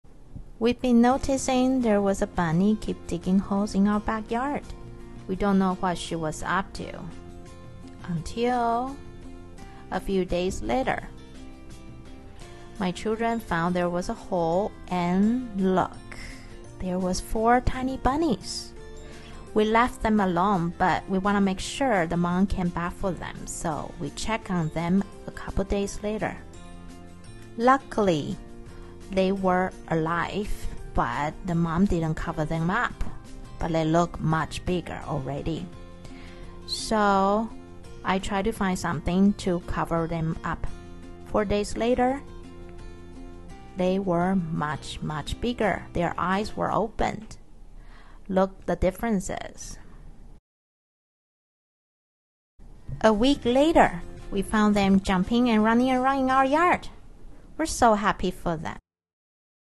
Wild baby bunnies in our yard🐰!! (babybunny cottontail 棉尾兔 )
We found a nest in our backyard with 4 baby bunnies!! We weren't sure if they were abandoned or not, so we looked up info. about what to do. We only checked on them to make sure Mommy Bunny came back to feed them and they were safe. They left their nest one evening, but ... they are still hanging around in our yard 🥰
我們在後院找到四隻棉尾兔寶寶！！我們不確定是否兔媽媽有回來餵食，所以我們查了許多資料。我們每天只有傍晚左右、確定兔媽媽有回來餵奶，確定他們是安全的。有一天晚上，他們長大離巢了 ...但是有時候，我們還是看到他們，在後院裡碰碰跳跳的呦！😊